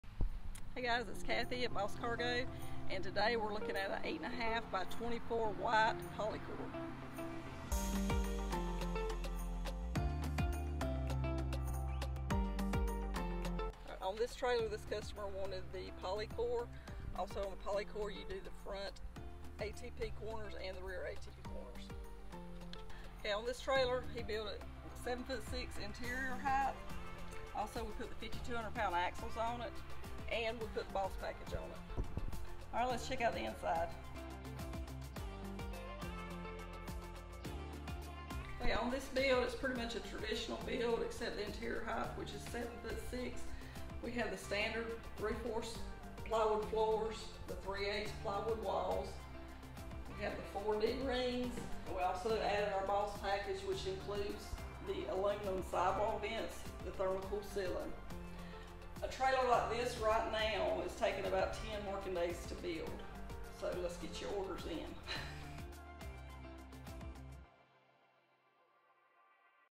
Brand New 2023 PolyCor Enclosed Cargo Trailer Walkthrough
Join Cathy on this BRAND NEW 8.5x24TA3 with POLYCOR SIDING!